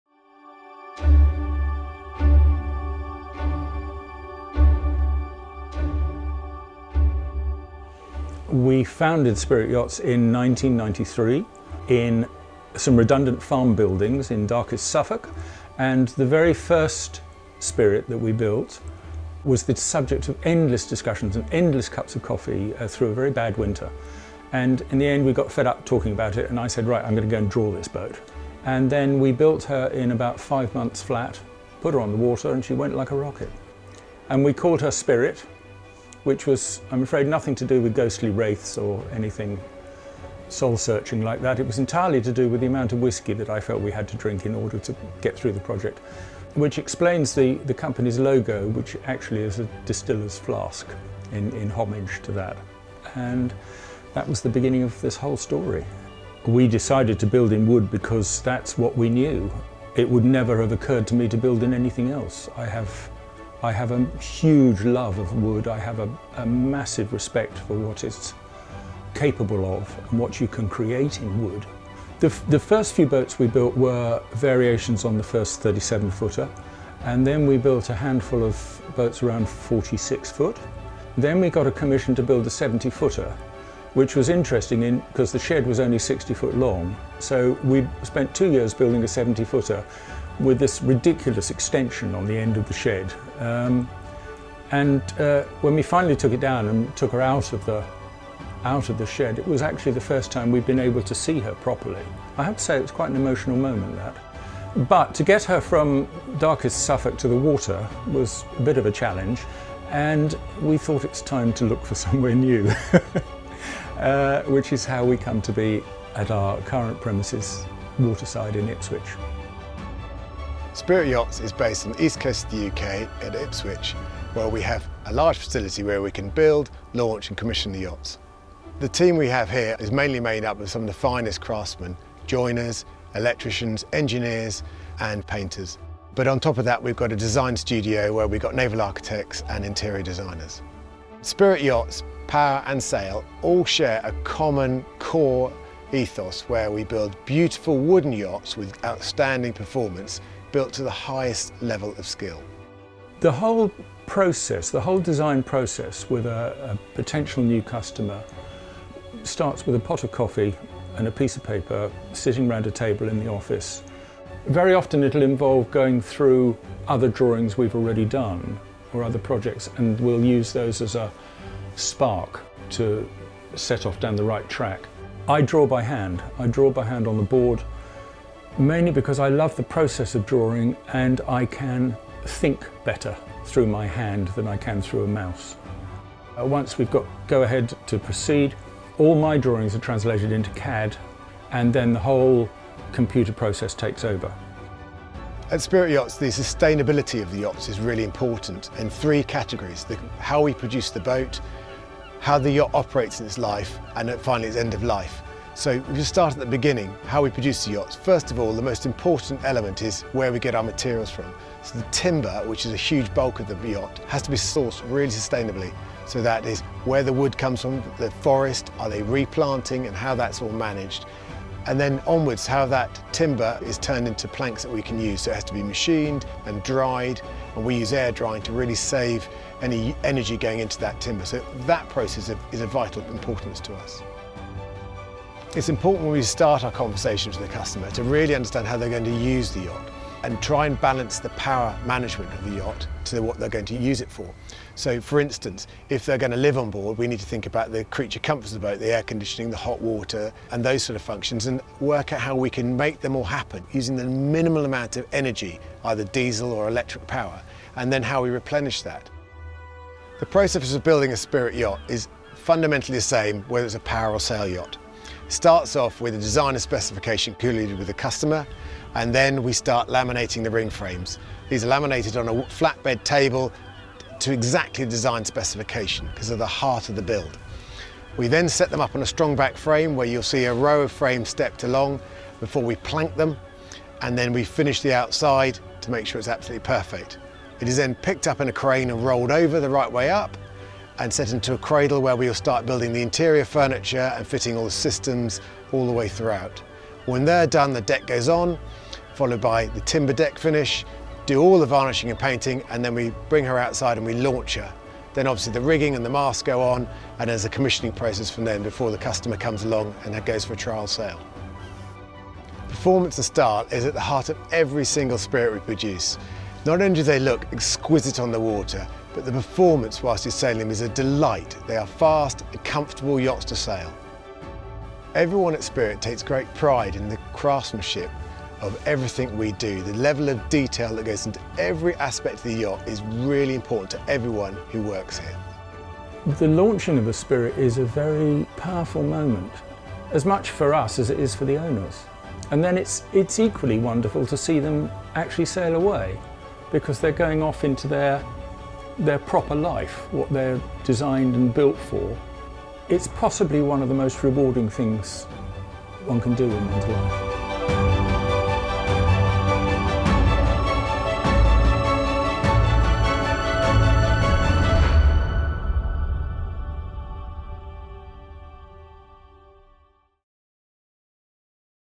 The Spirit Story
Everything you need to know about Spirit Yachts.  From a disused cow shed in rural Suffolk to a waterside facility launching world-class superyachts.  Since Spirit Yachts was founded in 1993, the company has been on quite a journey.

There have been Bond films, superyachts and eco-yachts, but the core ethos remains the same: beautiful, performance yachts hand-crafted from sustainable wood and enhanced by modern technology.

To really understand Spirit Yachts, you must first hear our story.